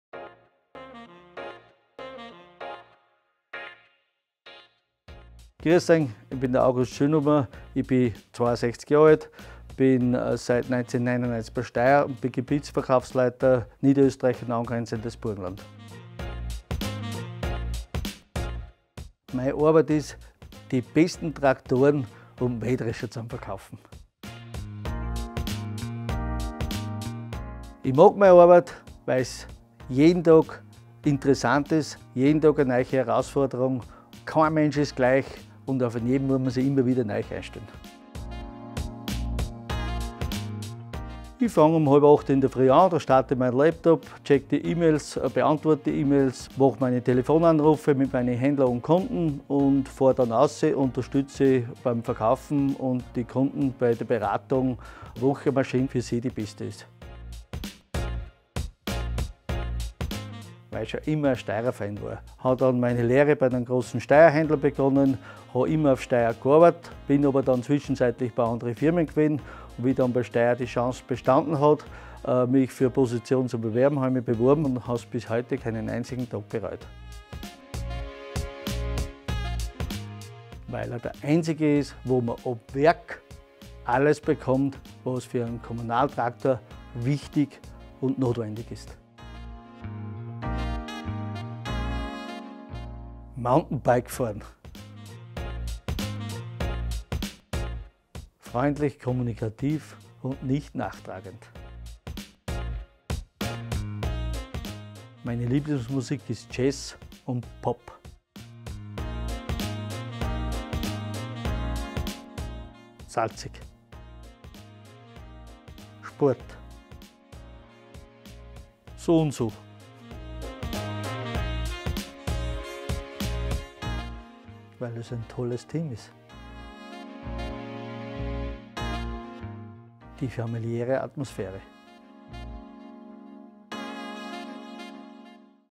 August Schönhuber - TEAM STEYR BACKSTAGE - ÖSTERREICH
Warum ein STEYR Traktor perfekt für den kommunalen Einsatz geeignet ist und welche Musik 🎶 er am liebsten hört, erzählt August euch im Video! 😁👆

August Schönhuber arbeitet als Gebietsverkaufsleiter für Niederösterreich und das Burgenland und gehört seit 1999 zum Team von STEYR Traktoren Österreich. 😊